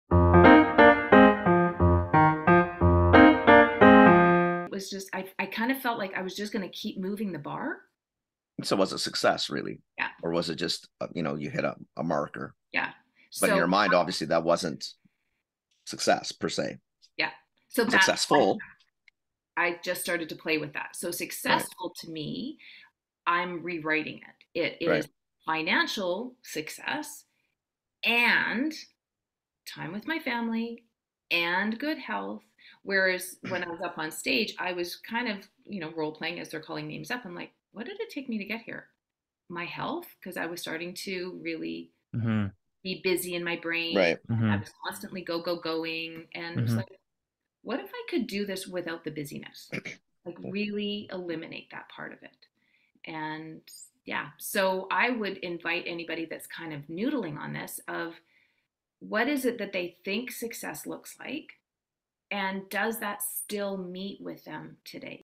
Rewriting what success looks like. Laurie Shopland takes a great business and blows it up
What would Laurie do? Actually, what did Laurie do? We talk to Laurie about the changes  she's made,  and she talks about her pivot and getting back into business with her husband Jim.